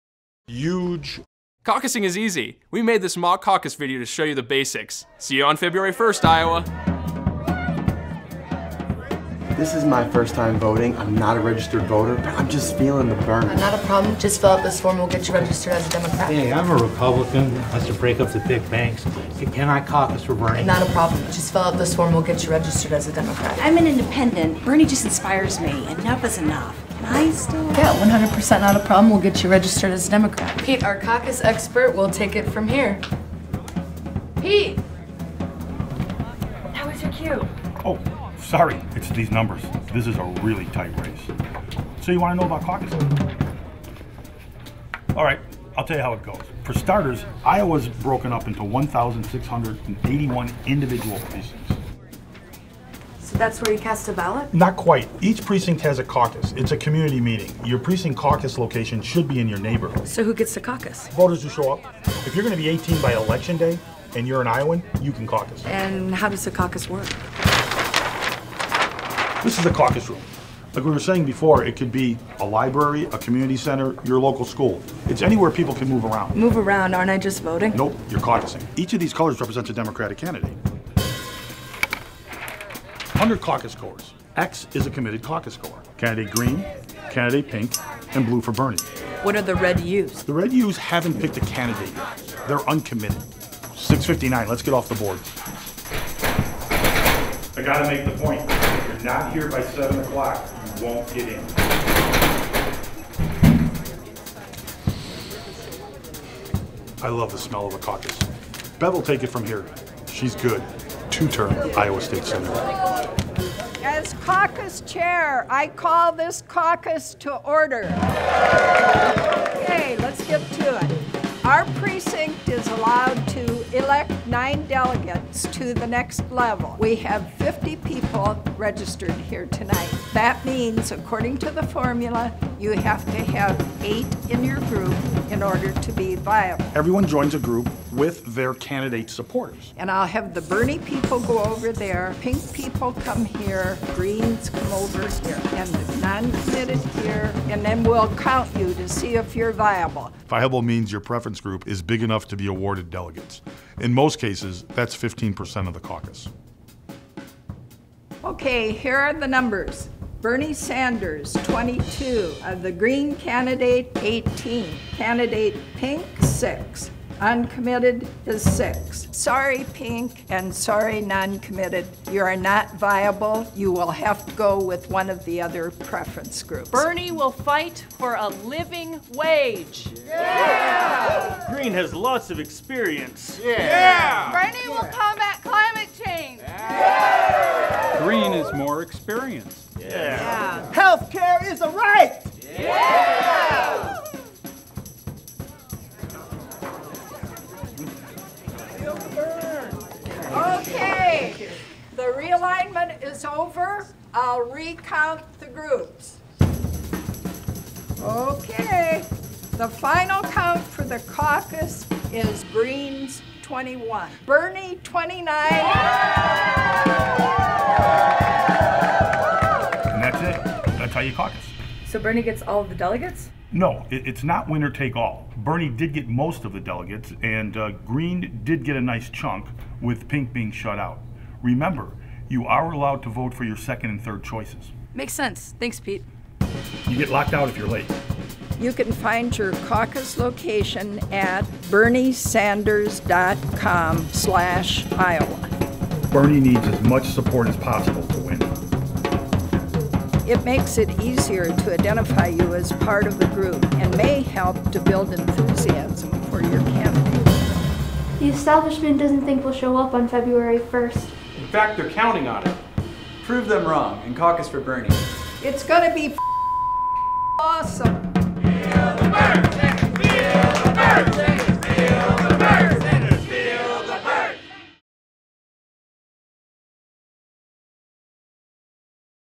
How To Caucus in Iowa for Bernie Sanders
Bernie's campaign released this video to show how to caucus for him in Iowa.  Please watch, wear Bernie stuff and bring your friends to the Iowa Caucuses on Monday, February 1st!